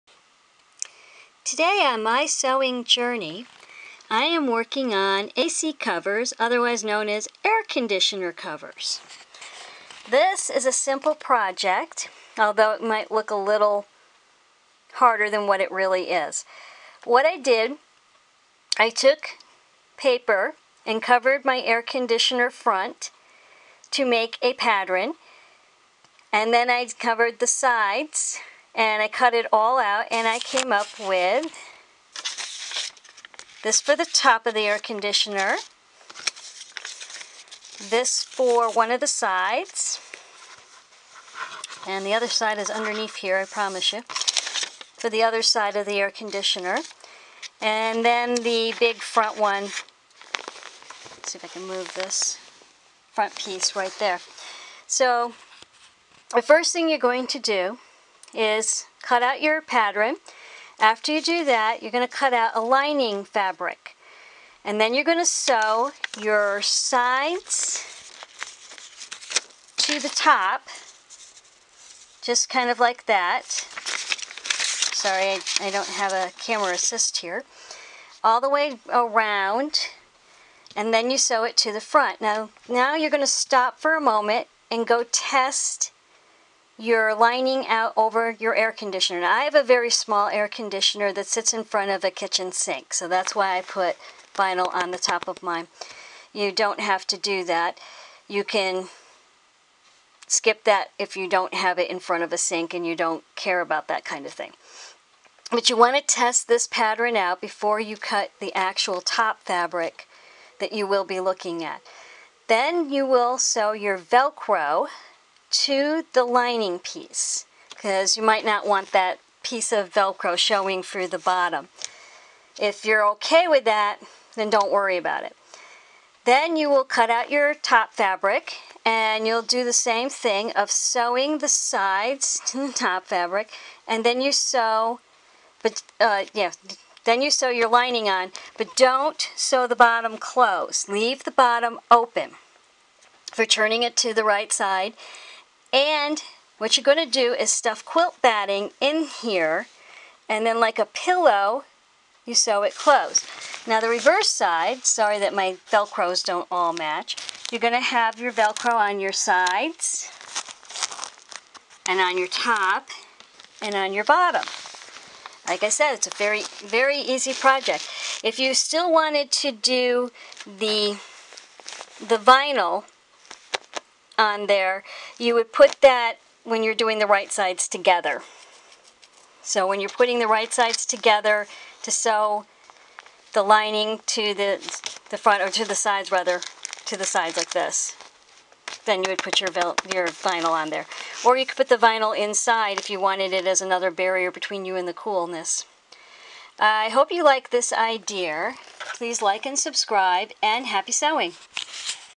A/C cover (Air conditioner cover)
Season 3 Episode 11  A/C cover (Air conditioner cover) This project is for the inside front of the a/c. It  can be glued if you didn't want to sew. This cover uses hook and loop tape, the tape is going on the a/c to attach it to the cover.  The cover is made from fabric.